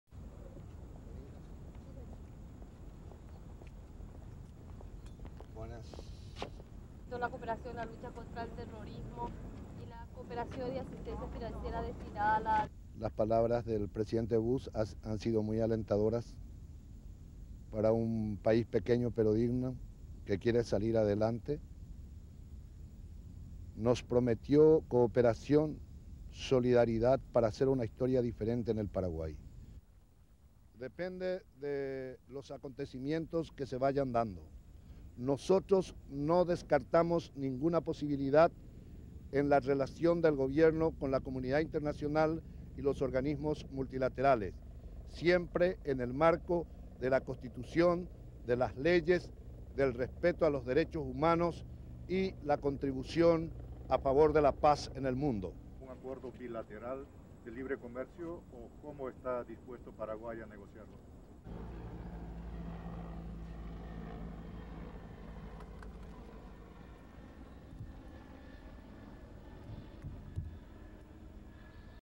Paraguayan president on cooperation with US
1. Paraguay's president Nicanor Duarte walking towards media
2. SOUNDBITE (Spanish) Nicanor Duarte, President of Paraguay
"President Bush's words were very encouraging for a small dignified country that wants to progress. He promised his cooperation and solidarity to make a different story for Paraguay."
3. SOUNDBITE (Spanish) Nicanor Duarte, President of Paraguay:
"We will not disregard the relationship of our government with the international community or multilateral organisations as long as they follow the constitution, the laws, respect human rights and there's a contribution to peace for the world."
4. Media
5. President Duarte walking away

STORYLINE:

Paraguay's President Nicanor Duarte was upbeat on Friday after his meeting with US President George W. Bush at the White House.

Duarte said the US President had offered many words of encouragement, including a promise of further cooperation between Paraguay and the US.

The President suggested that cooperation would come via financial institutions such as the International Monetary Fund, The World Bank and the Inter-American Development Bank, as well as an increase in direct help from the United States, particularly through freed trade.

The Paraguayan leader was speaking with reporters in the White House lawn, following a tour of the building.

President Duarte said Bush also expressed his "admiration" for efforts by Paraguay's government to fight against corruption and terrorism.